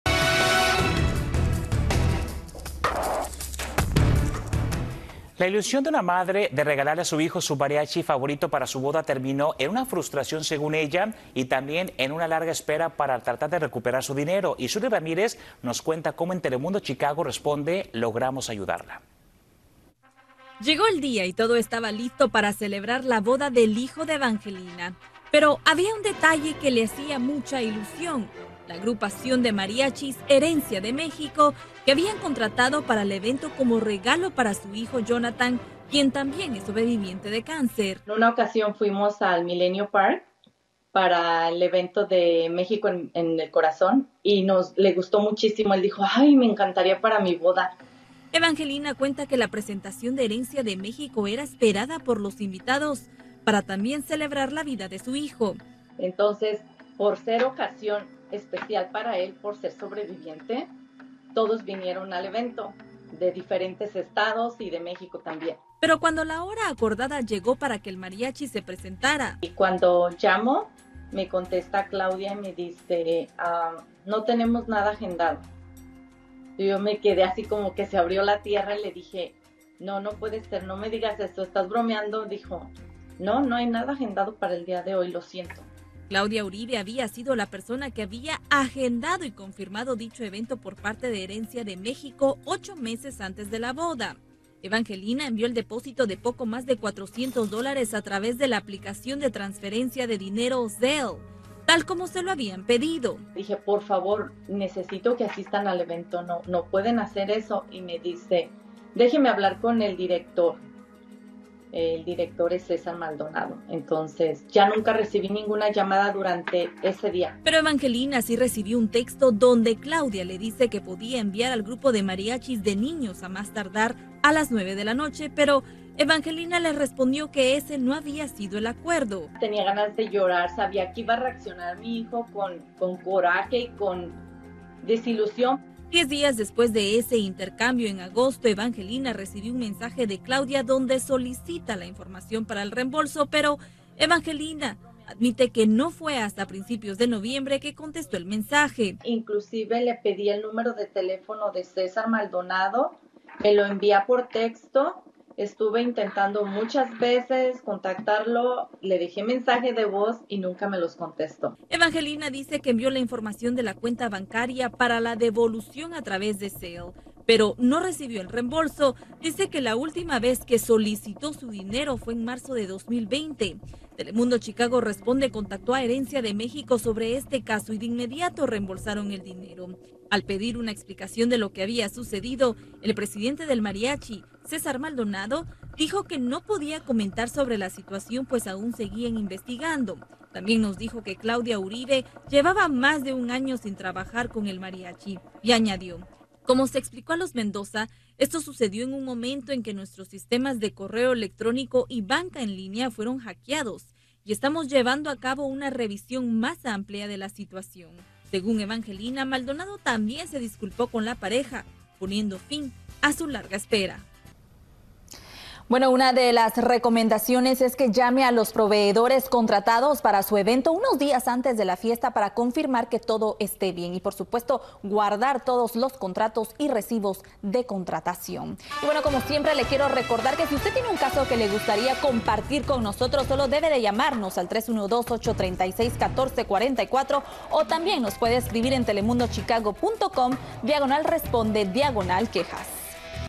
Contrató a mariachi, pero nunca llegó a la boda | Telemundo Chicago
La ilusión de una madre de familia de regalarle a su hijo una presentación de su mariachi favorito para el día de su boda, terminó en frustración y en una larga espera para recuperar su dinero.
Telemundo Chicago Responde.